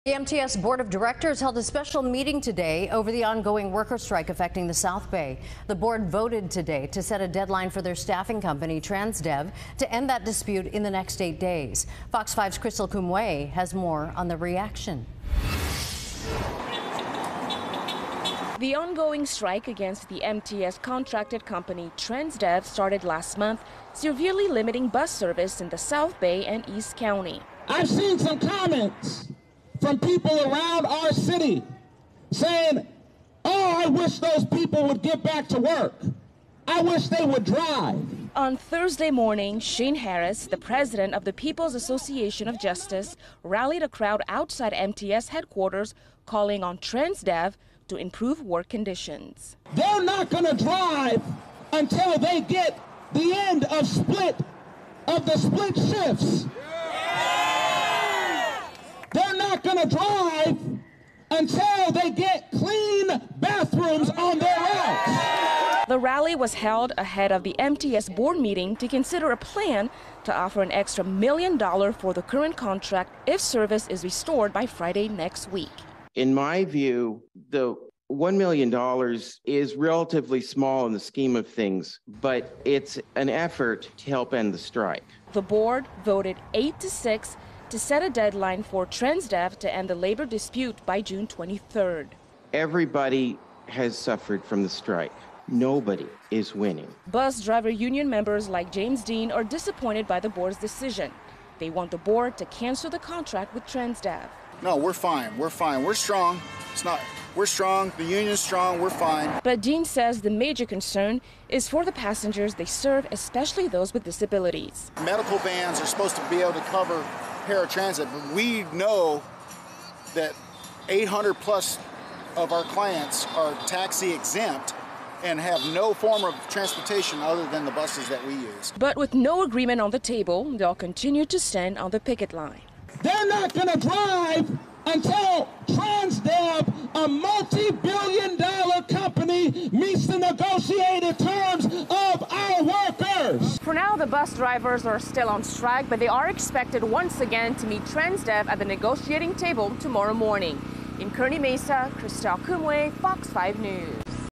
MTS Board Sets Deadline For Worker Strike
Christelle Koumoué reports.